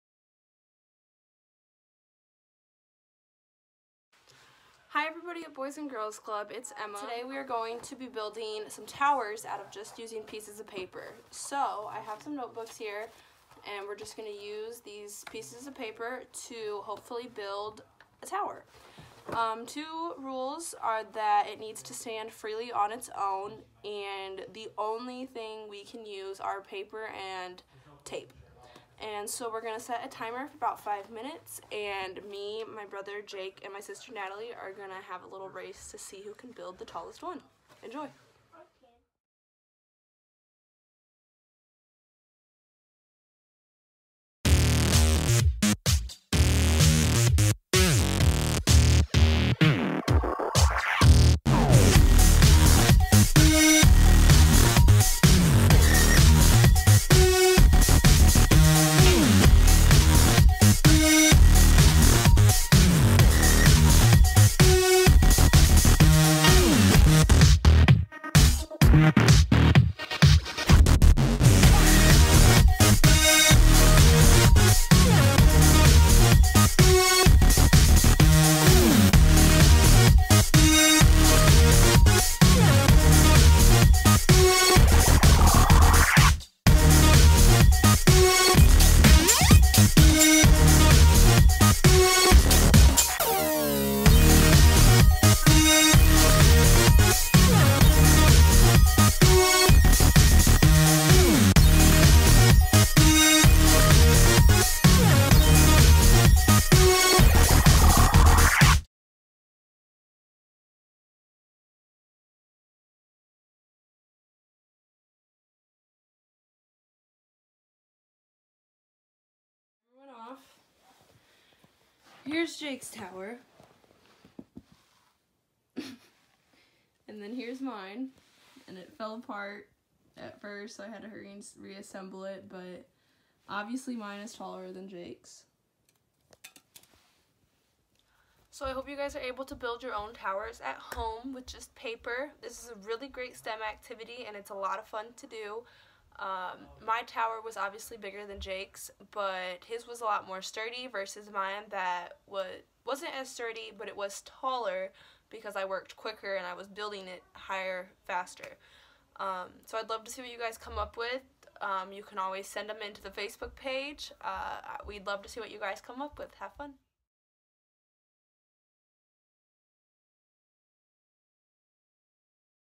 Paper Towers
Emma has a fun tower building challenge to teach the kiddos some basic engineering principals this morning!